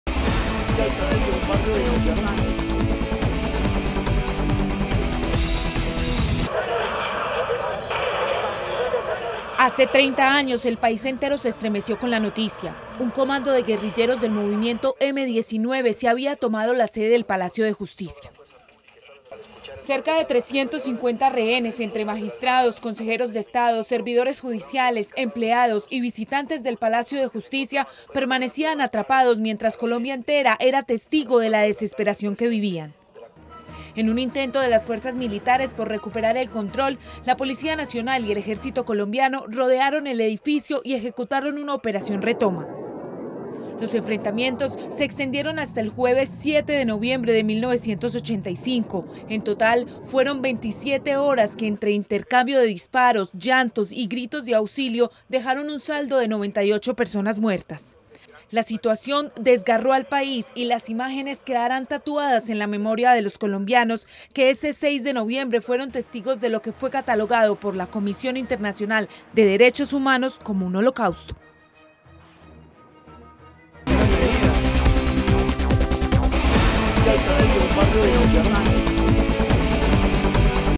Se Cumplen 30 Años De Holocausto de Palacio De Justicia| Teleantioquia Noticias
Hoy se cumplen 30 años de la toma del Palacio de Justicia. Grandes interrogantes se tejen alrededor de la suerte que corrieron varias de las personas que estaban ese día al interior de la edificación.

 En éste, su aniversario Teleantioquia Noticias les presenta este recuento historico.